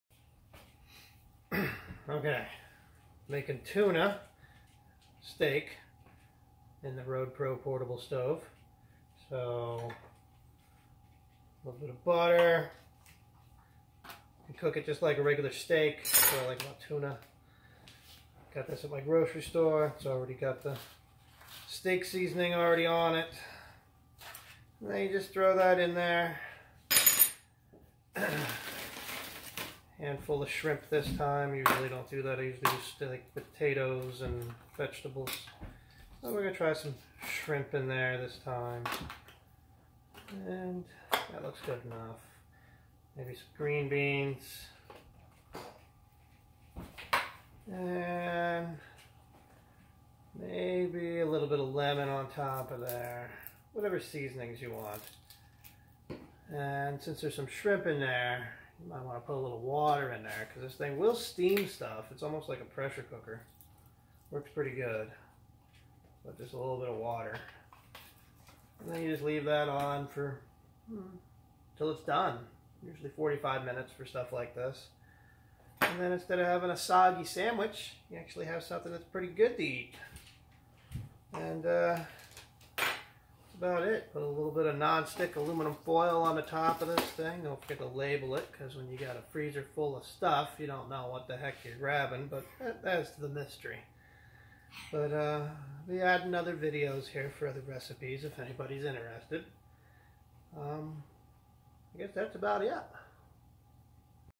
RoadPro portable stove food
tuna steak green beans and shrimp add a little butter whatever seasonings you want